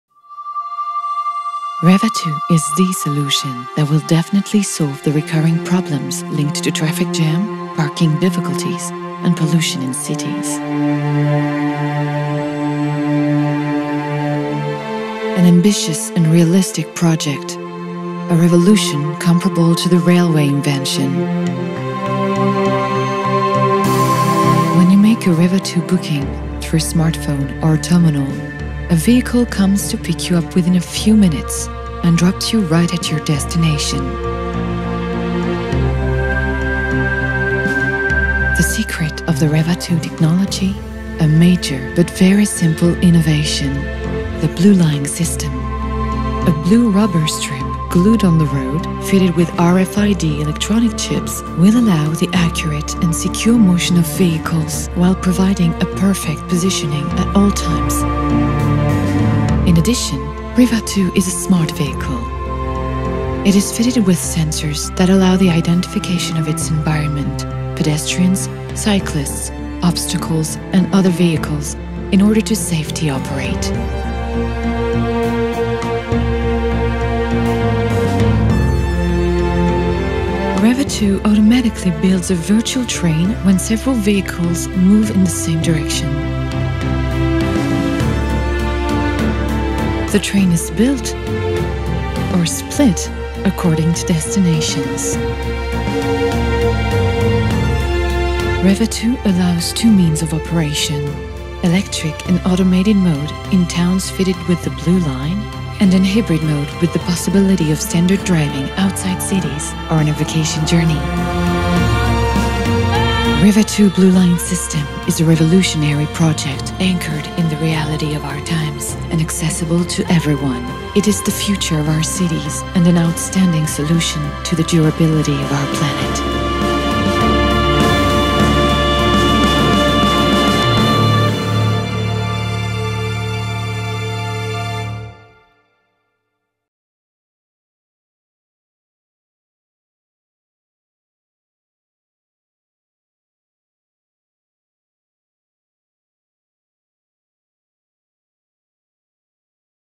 REVA2 BLUE LINE SYSTEM - US
Reva Blue Line System is a fully automatic urban commuting system. A simple phone call from your mobile phone and the vehicle picks you up to take you wherever you want, sharing your vehicle if you wish.